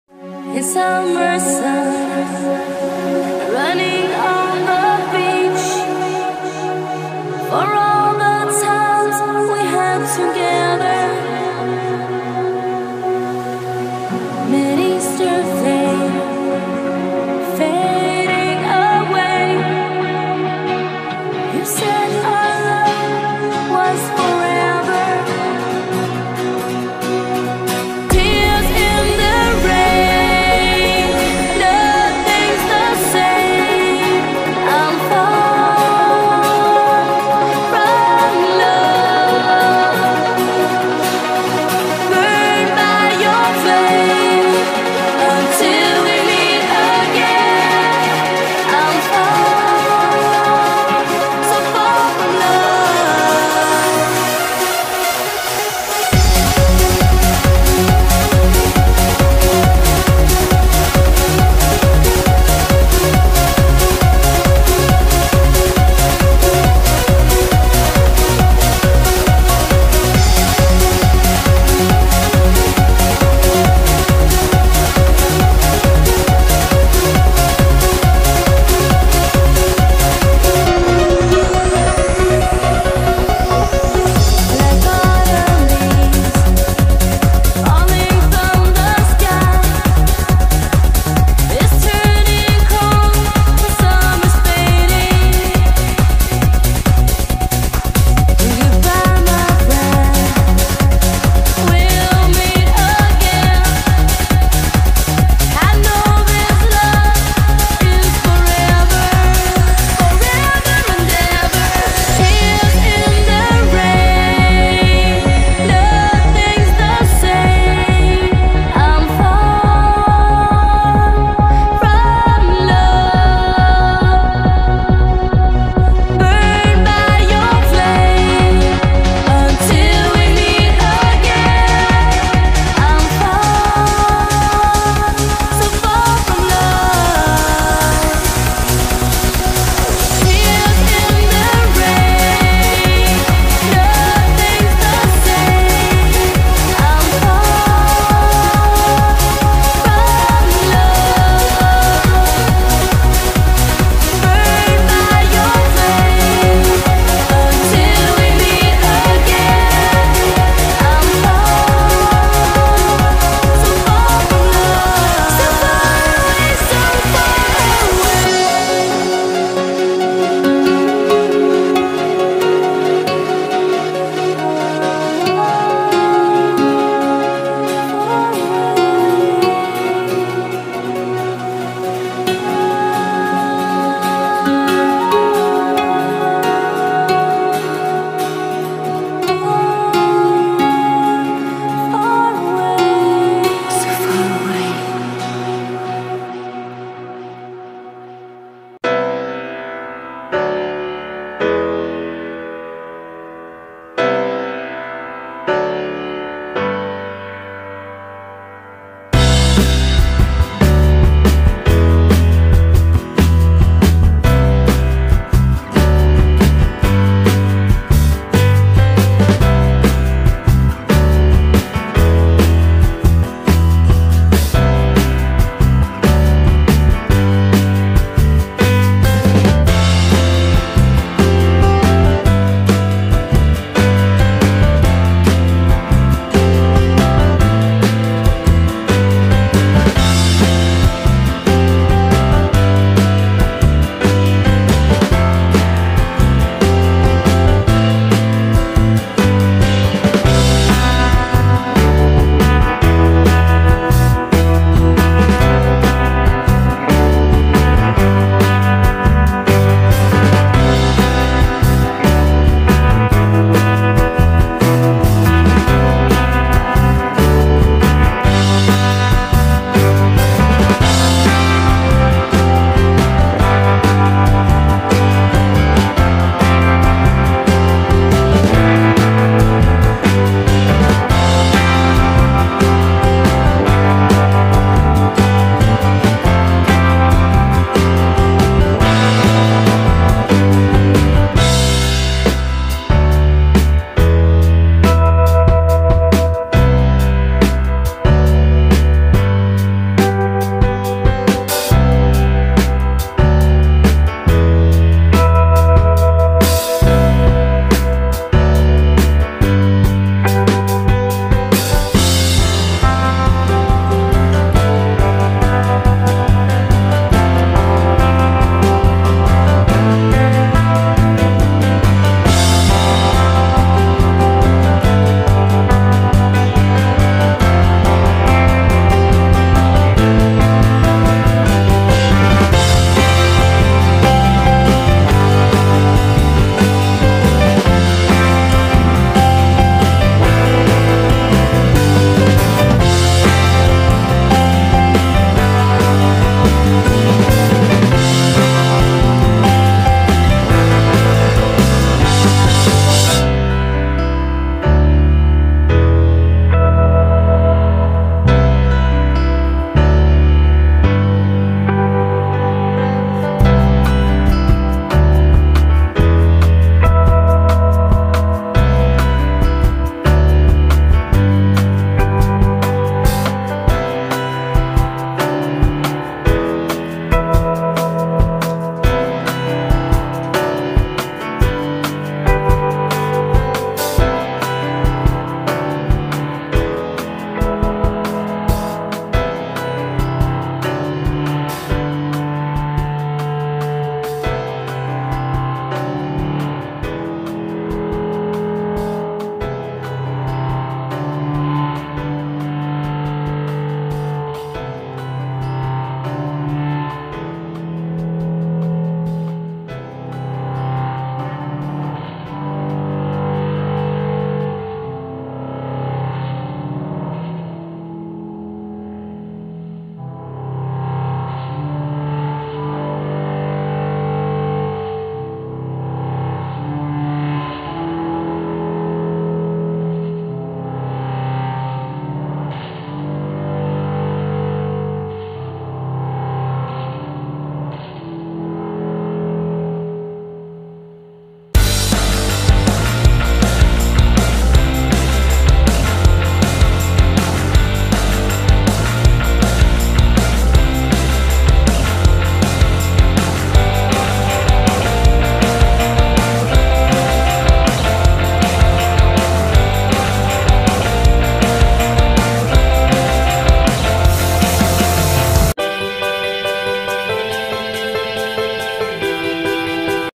🙏非常感謝天主、耶穌、聖母 : 我已經吃中式午餐太耐了，今日有機會吃西式，很開心及滿意。我的 “芝士粟米香煎雞扒+香腸仔、羅宋湯” 非常好吃，不過意粉稍多吃不完，分兩次吃。✌
PowerDirector